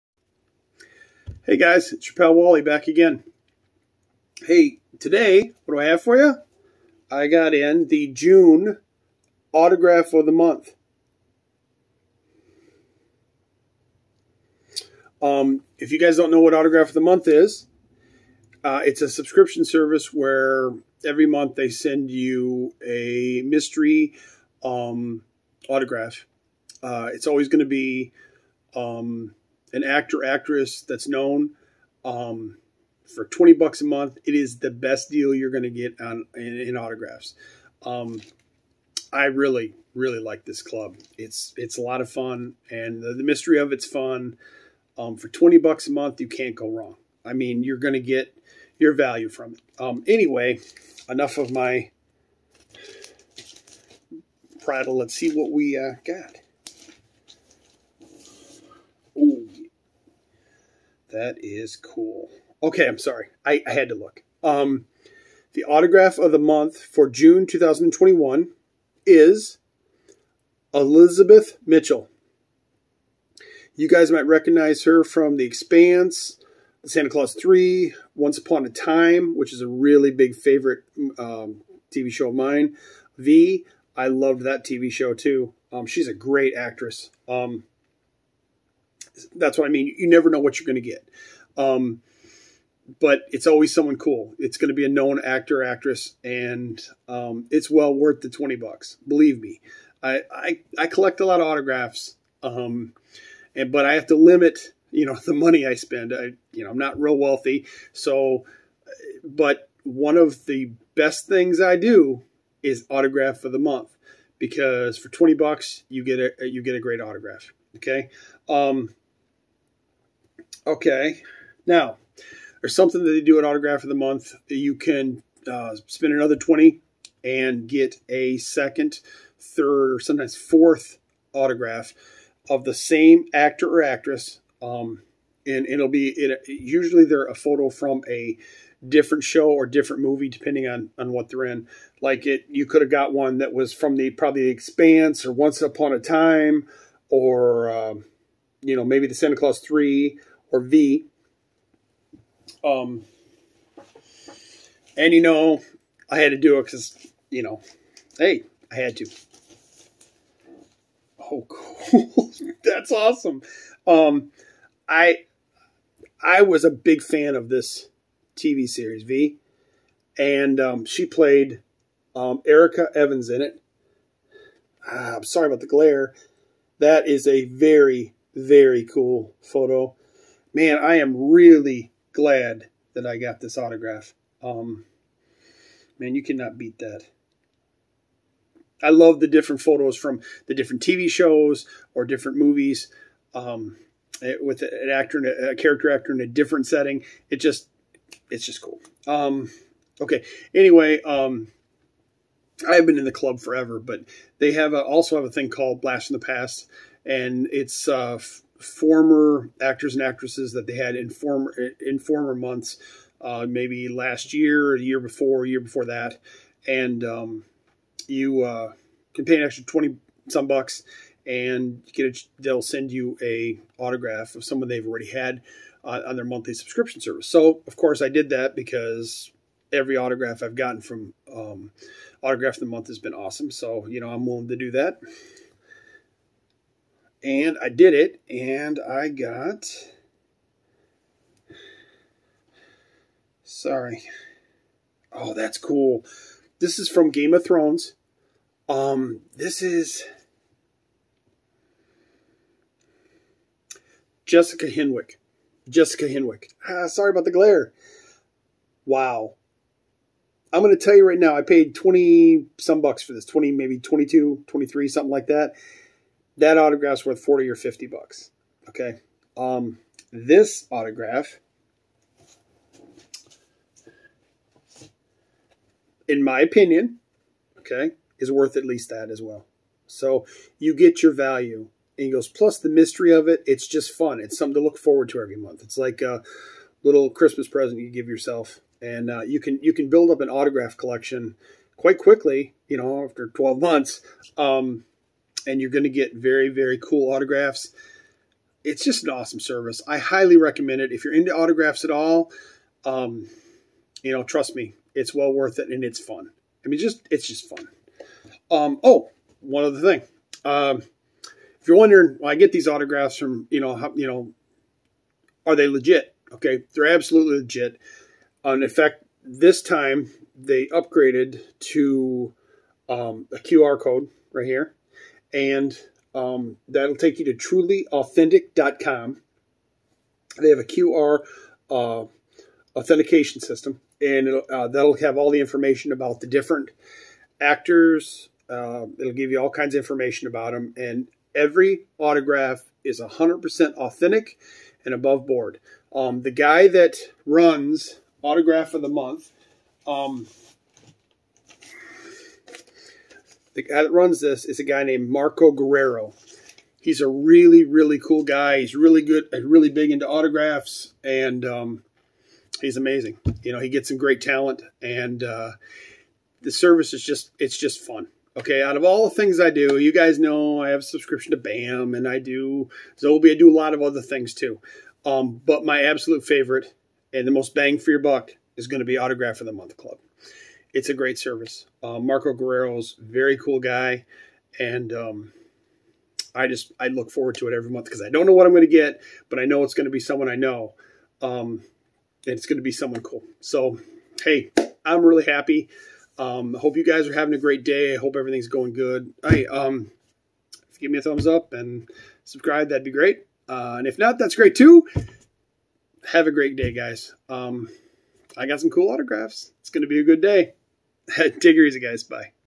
Autograph of the Month Club Review and Unboxing for June 2021
This is the June unboxing of autograph of the month with an extra autograph and a blast from the past extra autograph.